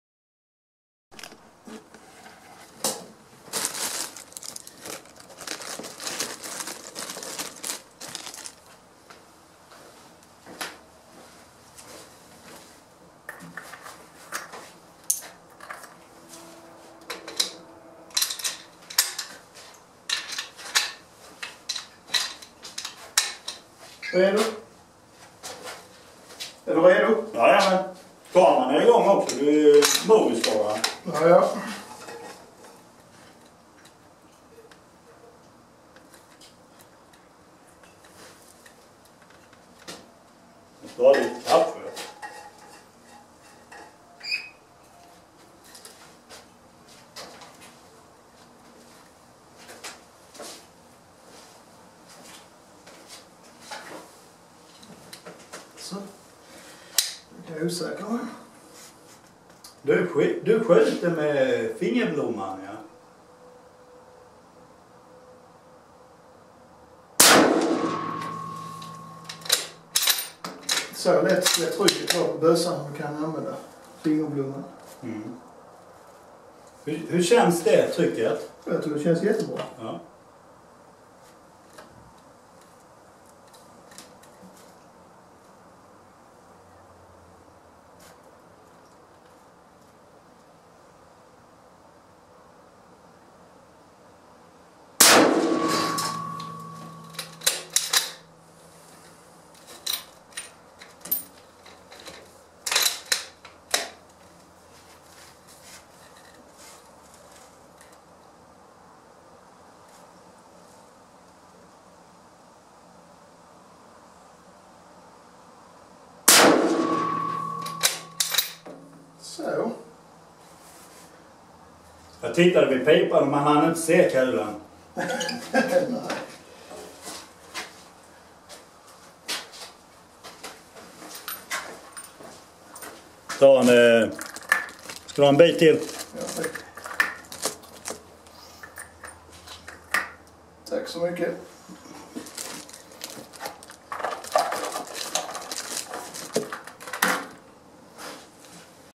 Sockerkaka med inskjutning
Johan Berglund gav ett andra utlåtande om bössan. Kikaren ramlade av så jag var tvungen att köpa nya ringar.